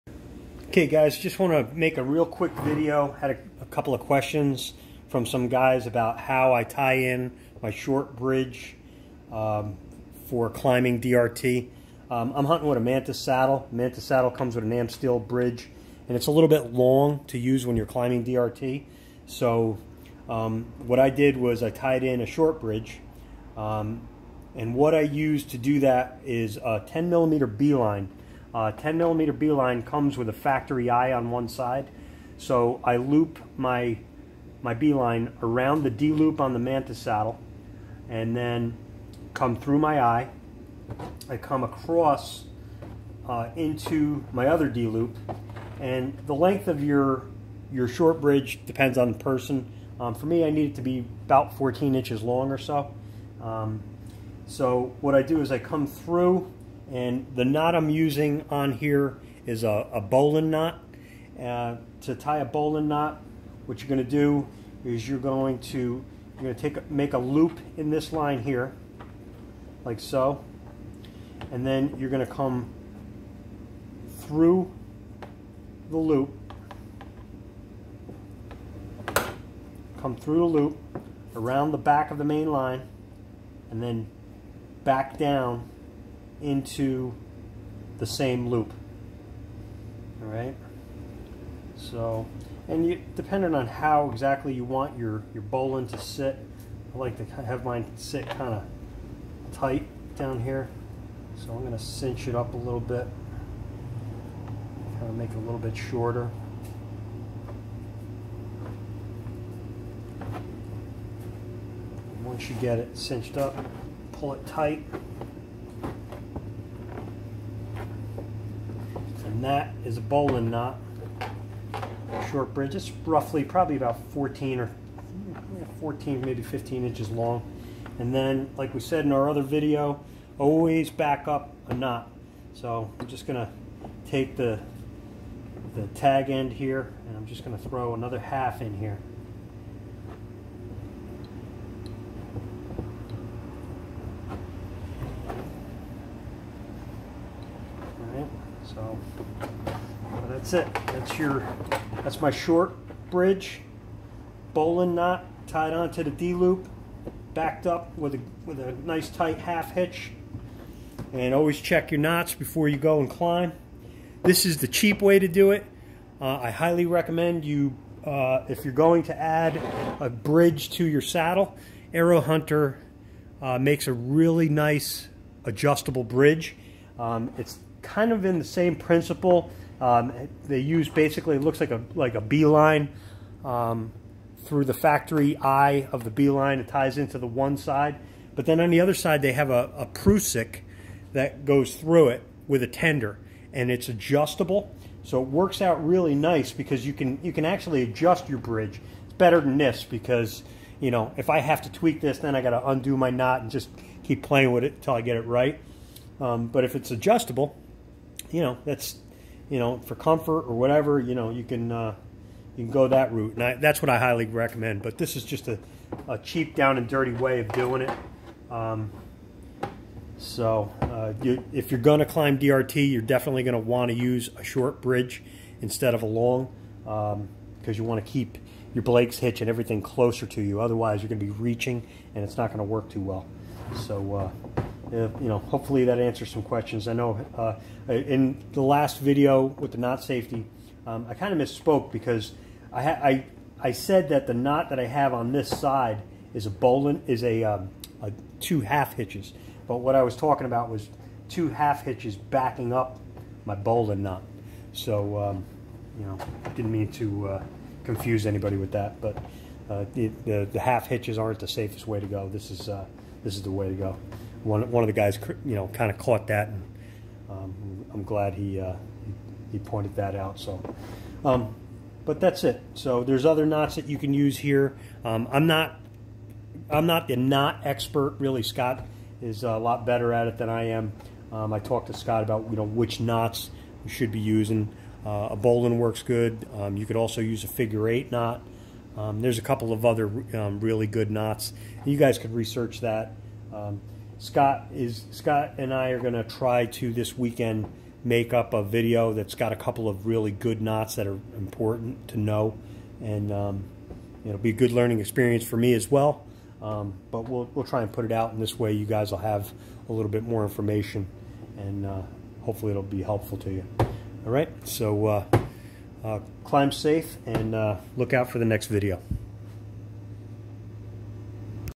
How I installed a short bridge for climbing DRT
Quick demonstration  on how I tied in my short bridge. I used 10mm Bee line for my bridge   I used Bowline knot  backed up with a half hitch.

This is how I did mine however , I do not recommend this. I recommend you purchase factory bridge . My favorite is the aero hunter bridge that comes on the aerohunter kestral flex. You should inquire to buy one over making one.